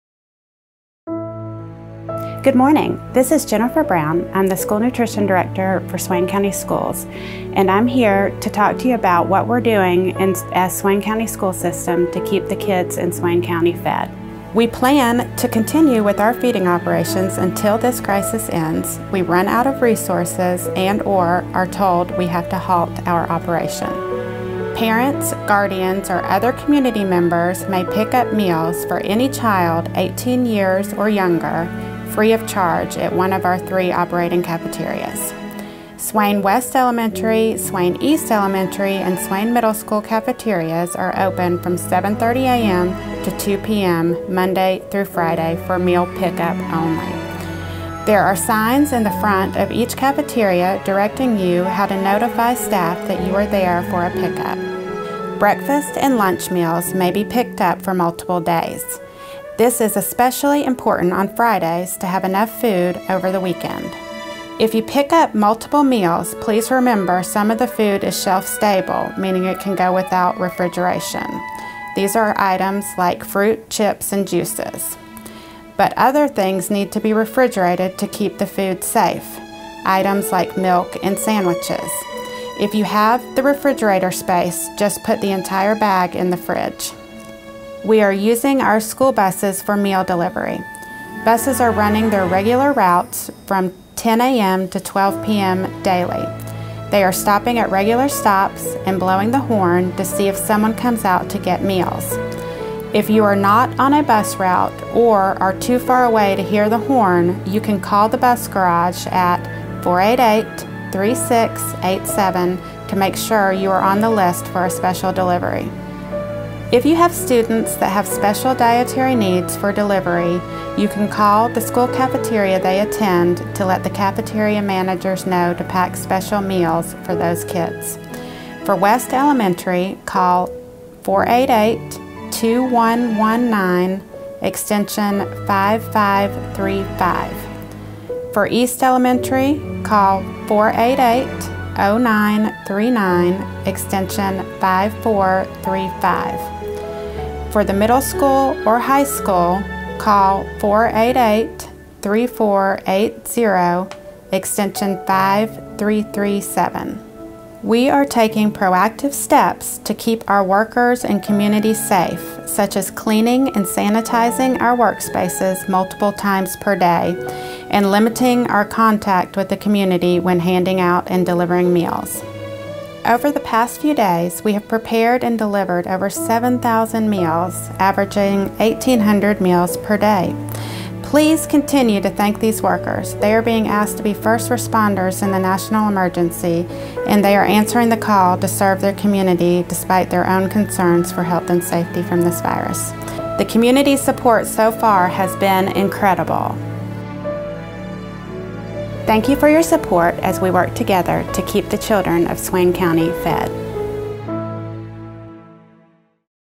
Special Meal Delivery for the Children of Swain County
School Nutrition Director for Swain County Schools, Jennifer Brown, tells us about the heroic efforts going on to keep the children of Swain County fed.